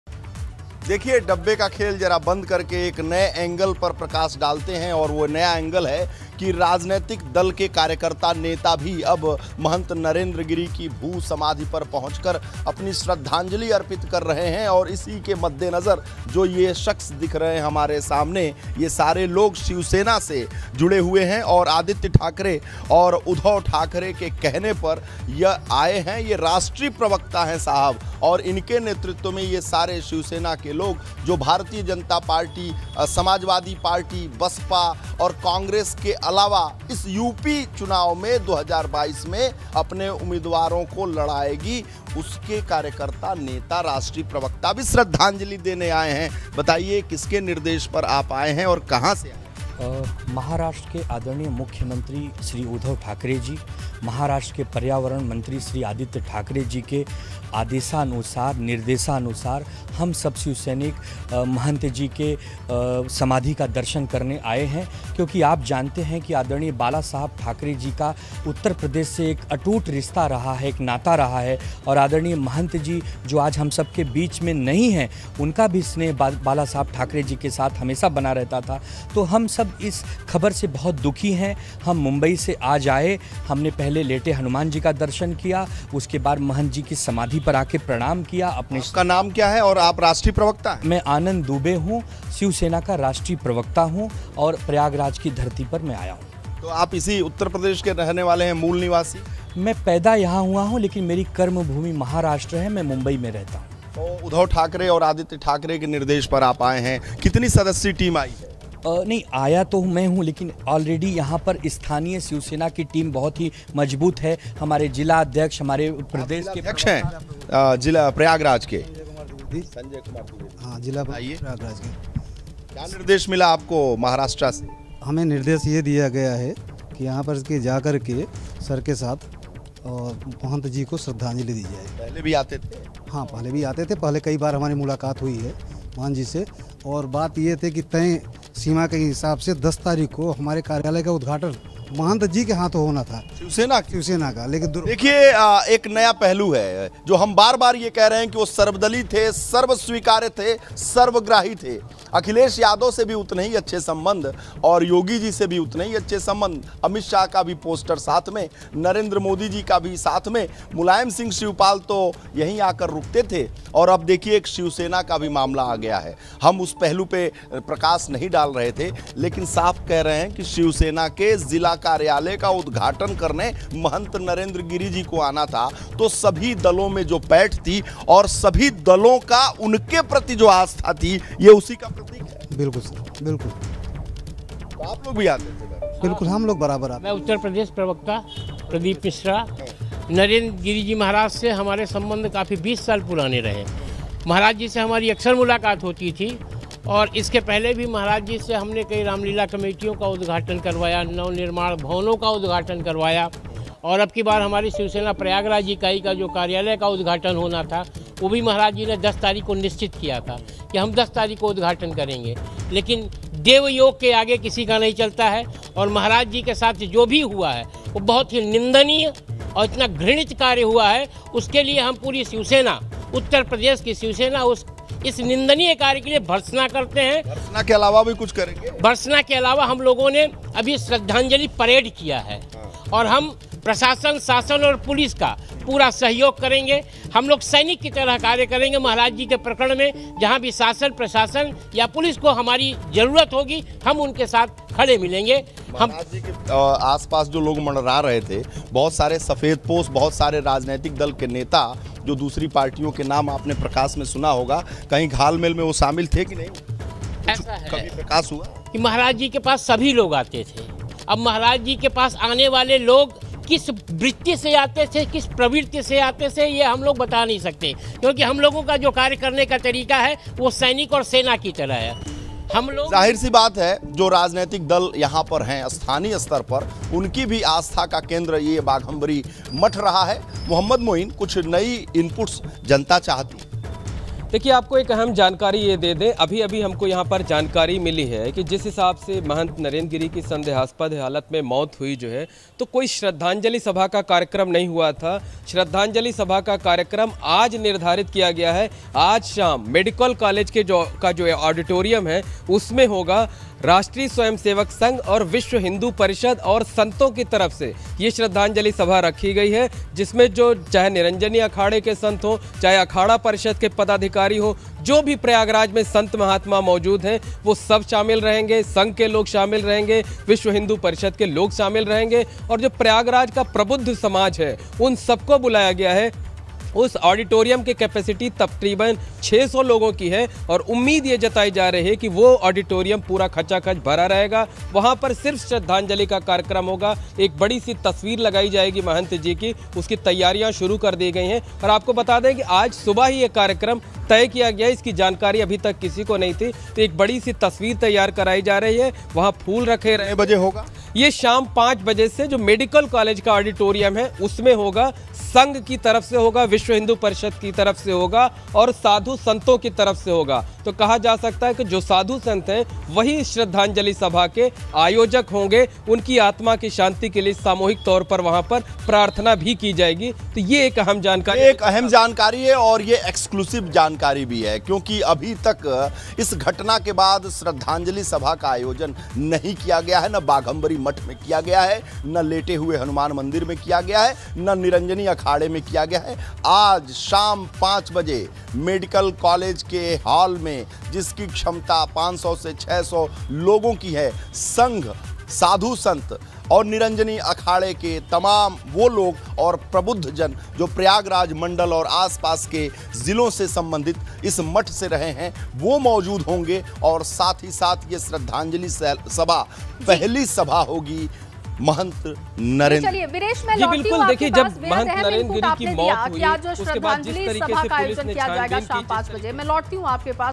Narendra Giri की समाधि पर श्रद्धांजलि देने पहुंचे शिवसेना के कार्यकर्ता, कही ये बात ! | Hindi News
राजनीतिक दलों के कई नेता महंत नरेंद्र गिरि की समाधि पर पहुंचकर श्रद्धांजलि अर्पित कर रहे हैं. इसी कड़ी में शिवसेना के कार्यकर्ताओं ने भी नरेंद्र गिरि की समाधि पर जाकर माथा टेका. साथ ही इन कार्यकर्ताओं का कहना है कि ये लोग ठाकरे के कहने पर यहां आए हैं.